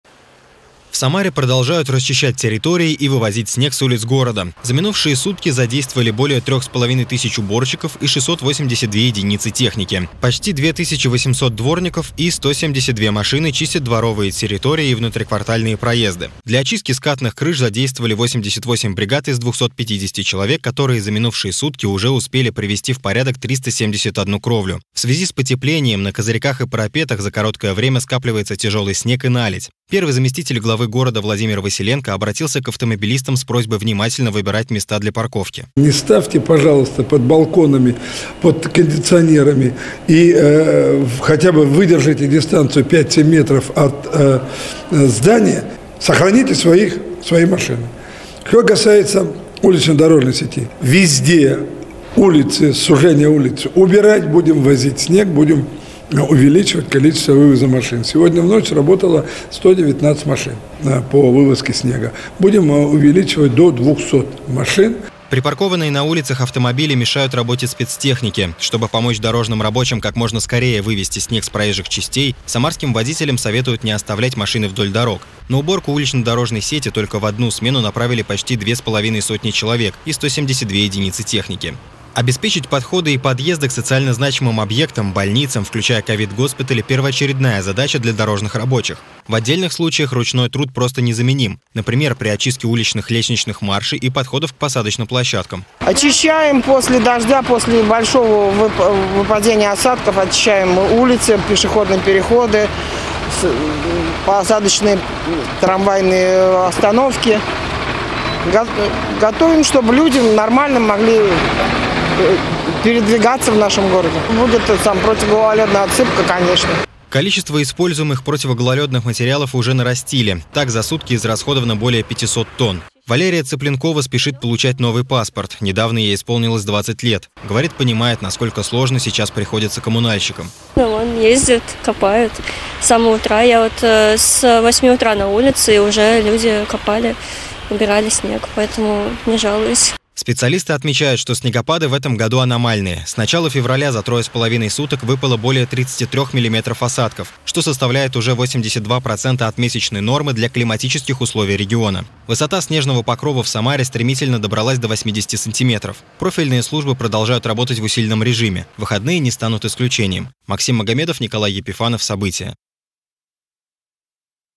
В Самаре продолжают расчищать территории и вывозить снег с улиц города
Погодные условия в Самаре за ночь резко поменялись, обильный снегопад сменился дождем. По данным синоптиков, в течение первых дней февраля уже выпала почти месячная норма осадков. Как проходит работа по уборке снега с улиц города расскажет Максим Магомедов.